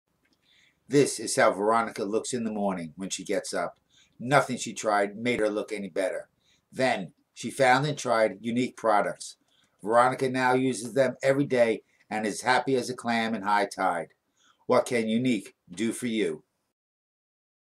Younique Looking
Younique looking video showing how Veronica King looked in the mornings until she found and started using Younique products.